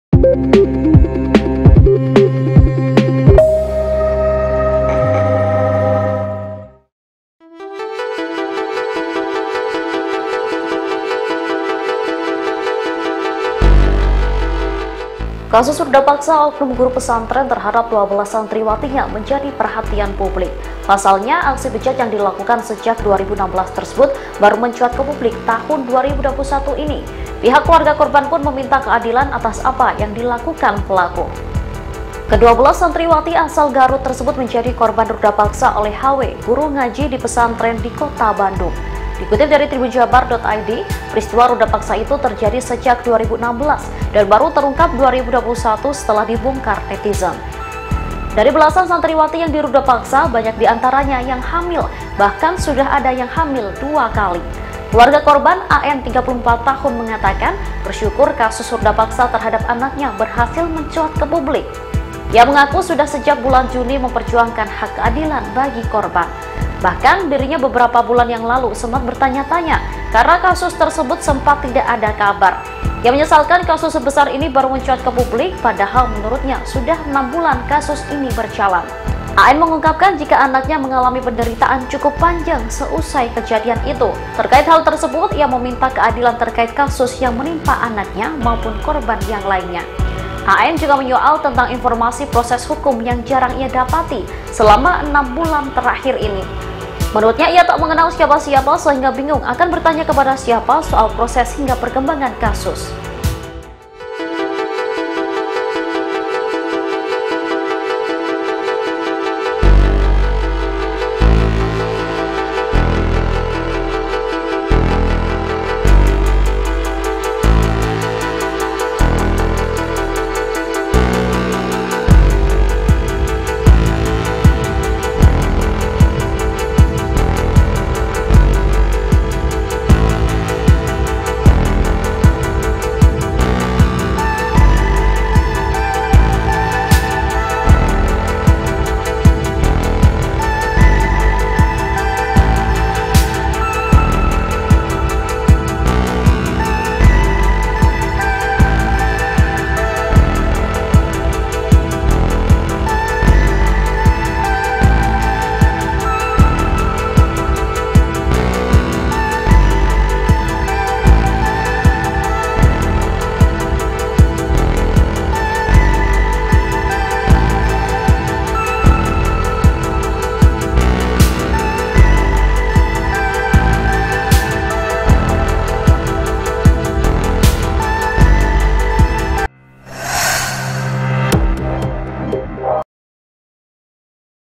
Kasus Guru Pesantren di Bandung Rudapaksa 12 Santriwati, Keluarga Korban Bersyukur Mencuat ke Publik
Kasus Rudapaksa oknum guru pesantren terhadap 12 santriwatinya menjadi perhatian publik.

Pasalnya, aksi bejat yang dilakukan sejak 2016 tersebut baru mencuat ke publik tahun 2021 ini.

Pihak keluarga korban pun meminta keadilan atas apa yang dilakukan pelaku.

Ke-12 santriwati asal Garut tersebut menjadi korban Rudapaksa oleh HW guru ngaji di pesantren di Kota Bandung.

Dikutip dari TribunJabar.id, peristiwa Rudapaksa itu terjadi sejak 2016 dan baru terungkap 2021 setelah dibongkar netizen.

Dari belasan santriwati yang dirudapaksa, banyak diantaranya yang hamil, bahkan sudah ada yang hamil dua kali.

Keluarga korban, AN (34) mengatakan bersyukur kasus rudakpaksa terhadap anaknya berhasil mencuat ke publik.